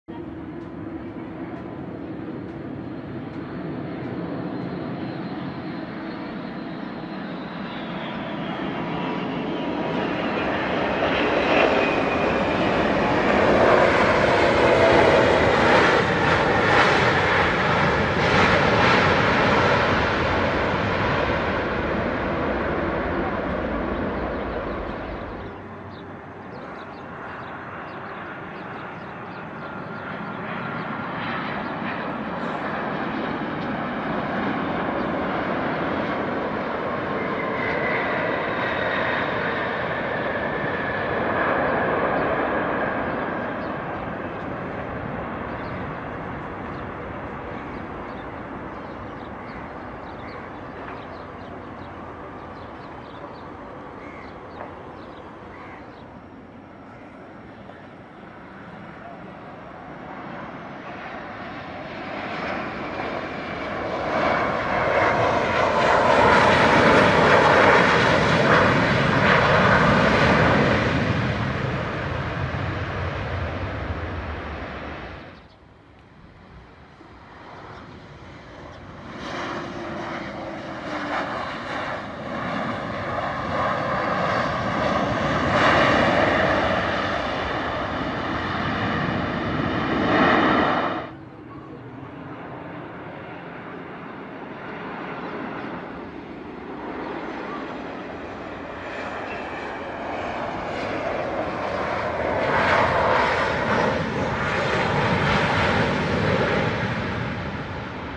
Dhaka 2018 - Smoky Speedy Hard landings
some of the noticeable landings shot  during 2018